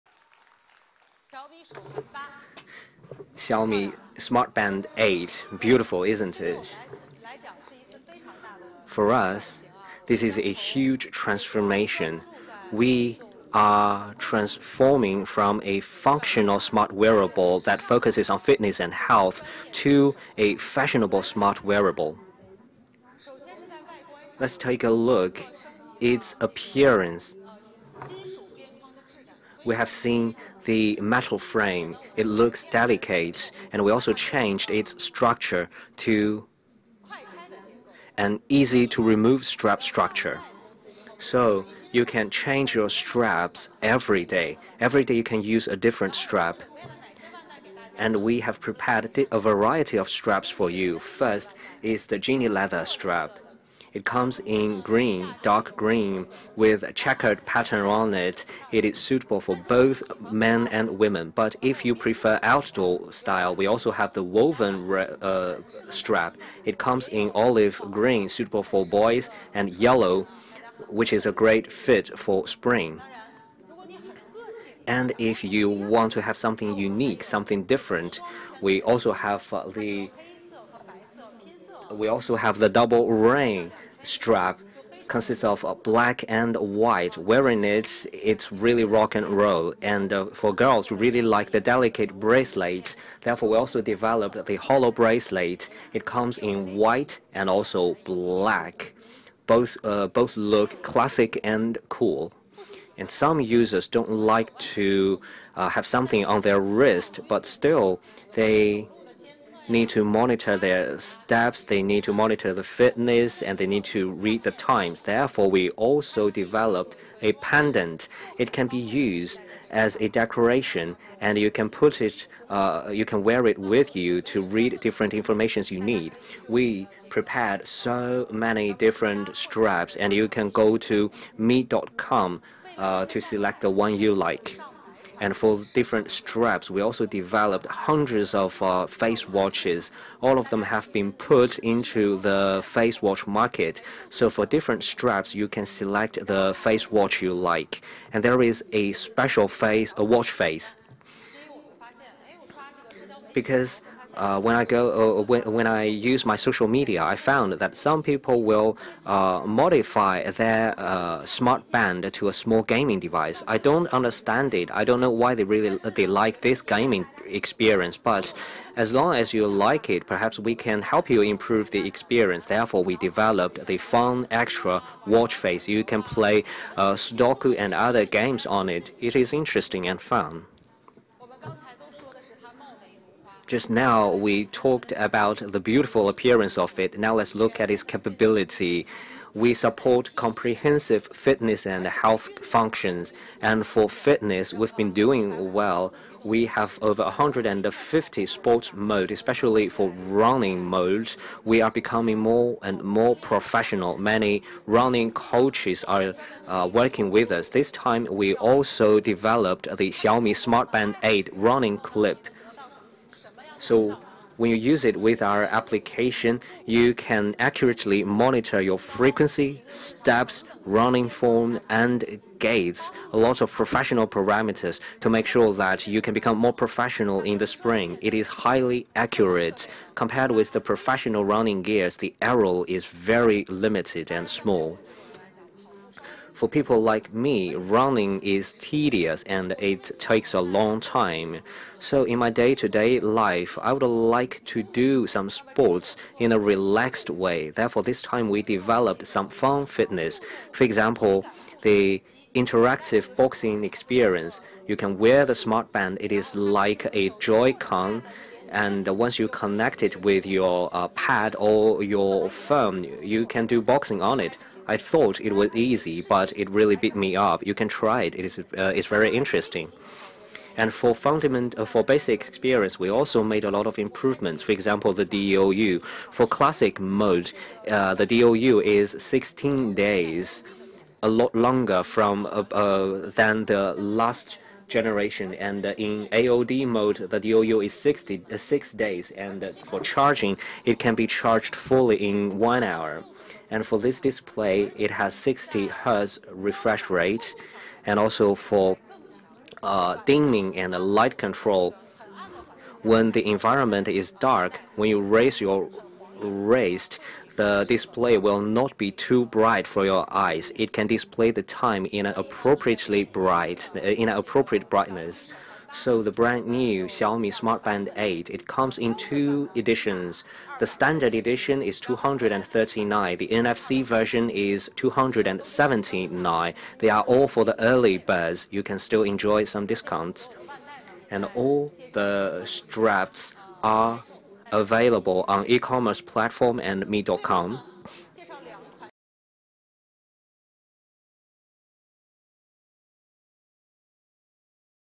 Xiaomi Unveils Smart Band 8
Check out the reveal of the the new wearable by Xiaomi.